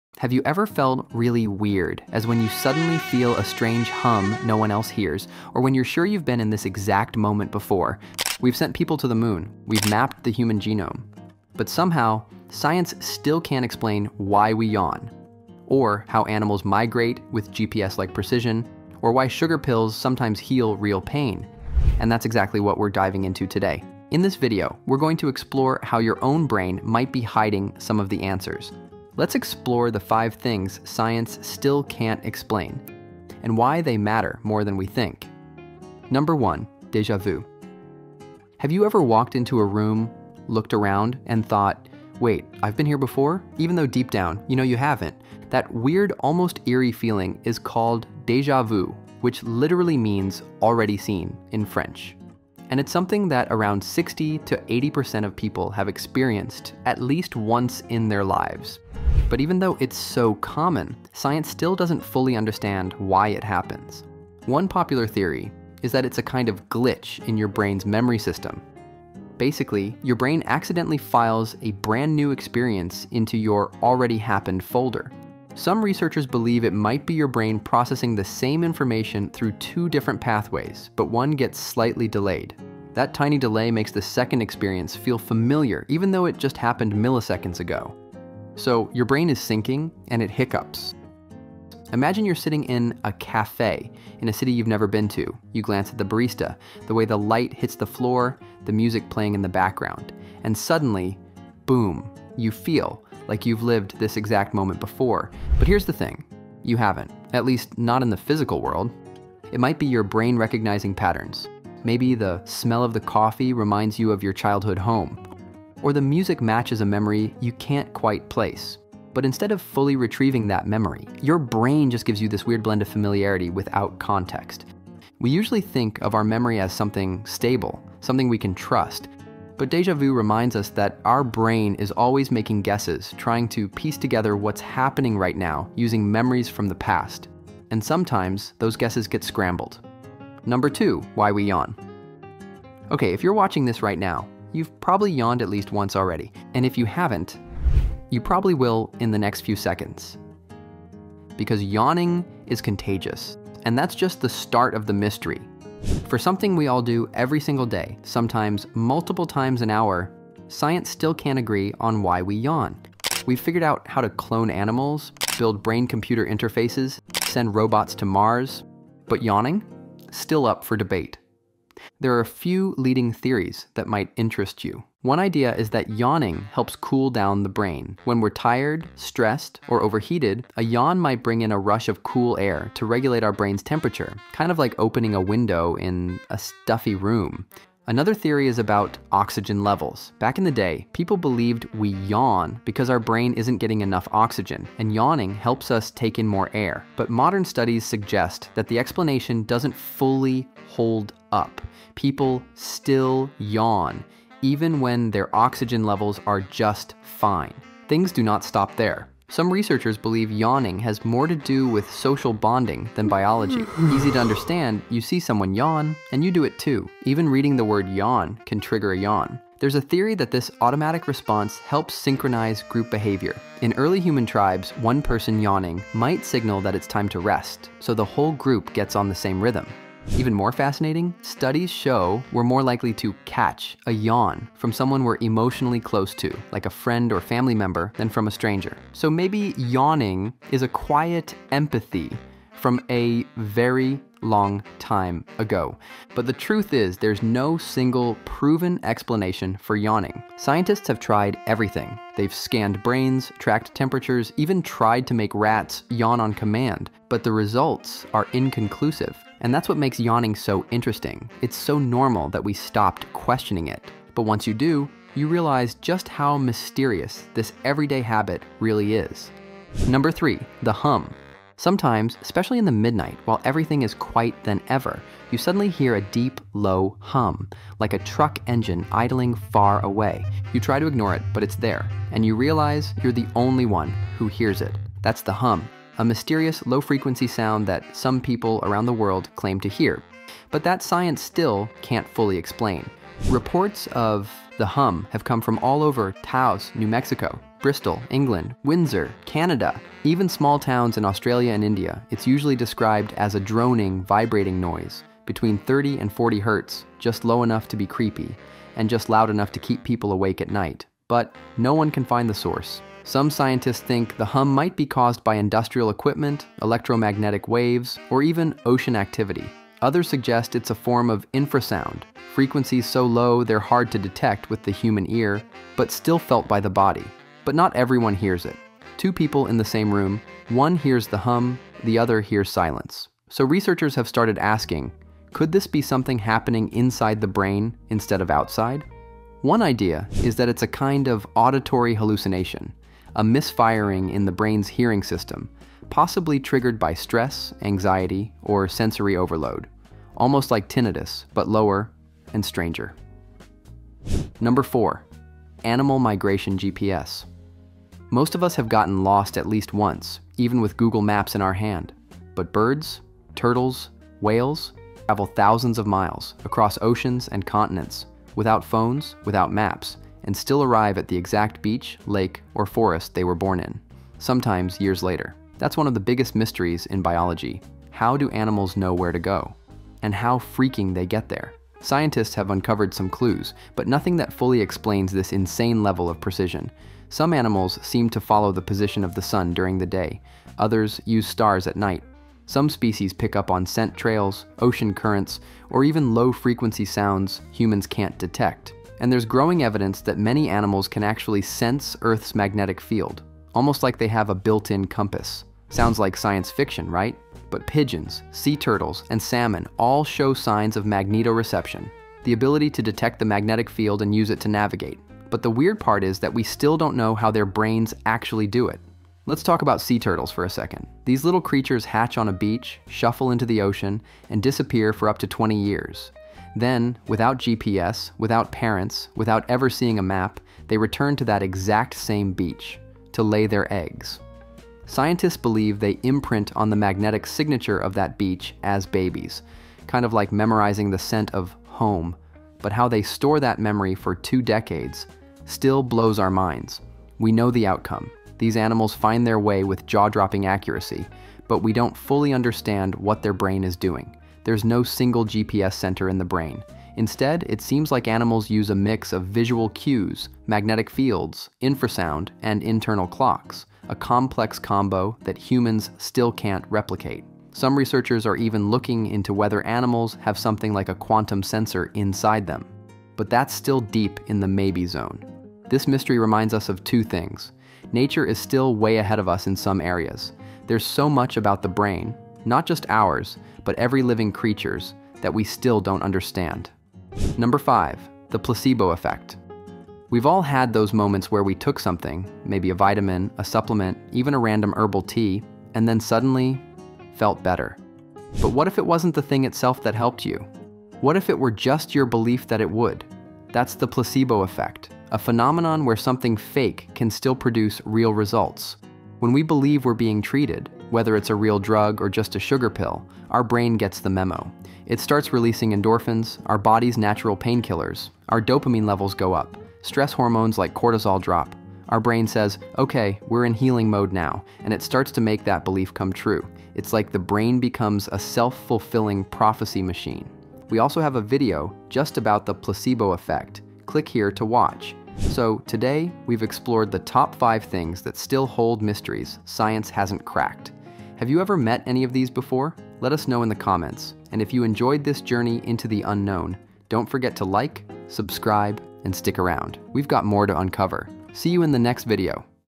5 Things Science Still Can't Explain
We’ve sent robots to Mars, split the atom, and even built AI cats like me… but there are still mysteries science just shrugs at. From everyday quirks to cosmic puzzles, here are 5 things that—even today—science still can’t fully explain